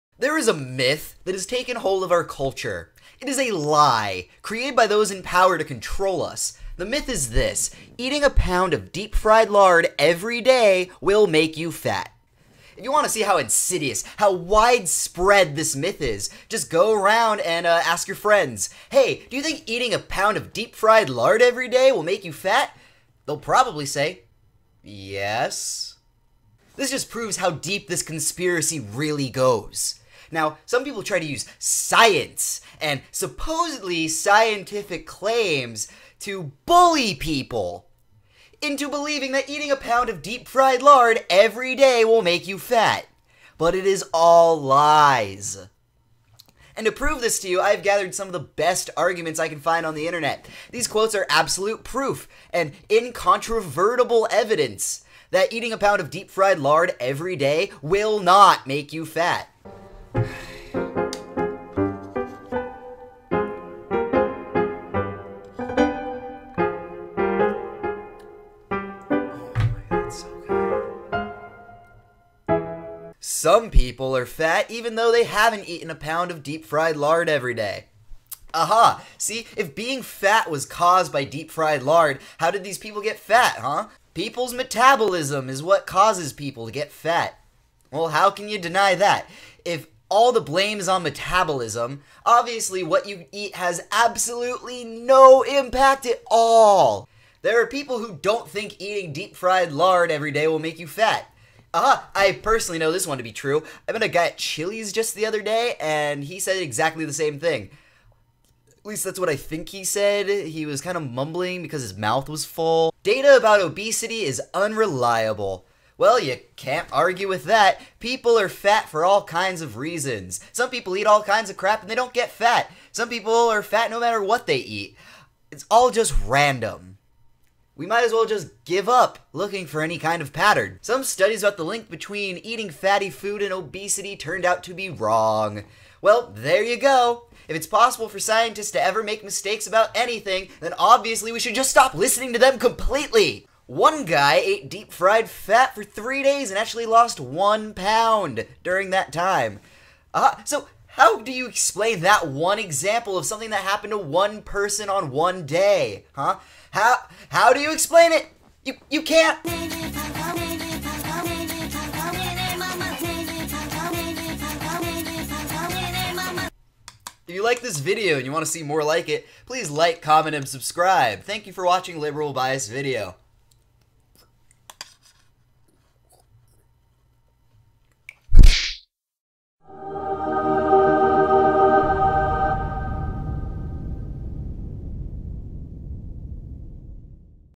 Ep 11: This video is about deep-fried lard and NOT ABOUT GLOBAL WARMING. Honest. No, really.
You should totally eat a pound of deep-fried lard every day. It will not make you fat. How do I know? Science has not proven that it will.

Zach patiently goes through all of the explanations that "Lard will make you fat"-deniers use to disprove the so-called "science" that some people think shows that eating tons of lard will make you fat. But obviously, that is all just a big conspiracy.

Some people might think that this video is secretly a metaphor for arguments against climate change. That's just silly. St00pid liberals.

Watch Zach as he COMPLETELY DESTROYS ALL OF THE SO CALLED SCIENCE behind the idea that eating deep-fried lard will make you fat. You can eat whatever you want and still be skinny. You can eat anything you want, not exercise, and not change your diet, and still lose weight and stay lean and ripped and toned.

How does Zach know that this is true? Here are some of the intelligent and compelling arguments that he uses:

“Some people are fat, even though they haven’t eaten a pound of deep-fried lard every day.”
“People’s metabolism is what causes people to get fat.”
“There are people who don’t think eating deep-fried lard every day will make you fat!”
“Data about obesity is unreliable.”
“Some studies about the link between eating fatty food and obesity turned out to be wrong.”
“One guy ate deep-fried fat for three days, and actually lost 1 pound during that time!”

So as you can plainly see, there is absolutely no reason to believe that eating deep-fried lard every single day will make you fat.

If you are STILL NOT CONVINCED, make sure you watch the whole video to understand Zach's full explanation.

[/satire]

For those of you dedicated enough who have actually read this far into the description (HI THERE!), the key term for today is: SKEPTICISM.  Skepticism is awesome, except when people who just don't understand science use "skepticism" as an excuse to dismiss facts, like human-caused climate change, that they don't like. Sometimes, you have to take people's arguments out of one context, and play them out in a different context, to see exactly how ridiculous they really are.